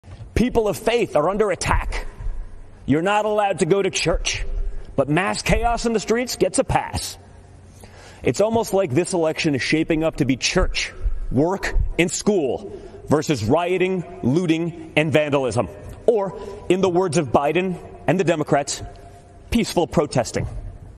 Donald Trump Jr.: on Joe Biden and Democrats supporting "rioting, looting, and vandalism"
Donald Trump Jr.: This election is shaping up to be church, work, and school vs. rioting, looting and vandalism